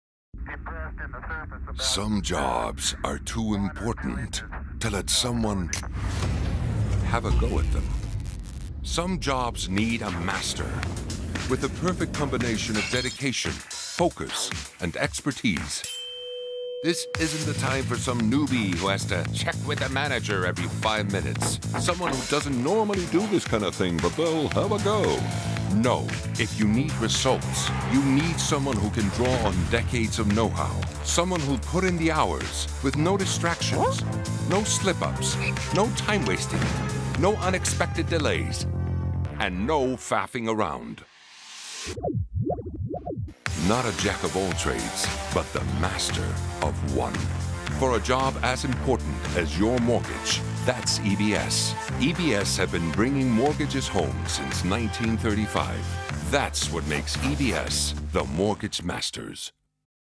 EBS - The Mortgage Masters
Thinking of a new home? It's the most important purchase you'll ever make, so you'll need the best in the business - like an EBS Mortgage Master. They don't claim to be the Jack of all trades, but they're pretty proud to be the Master of One. to meet your local EBS Mortgage Master. Ad.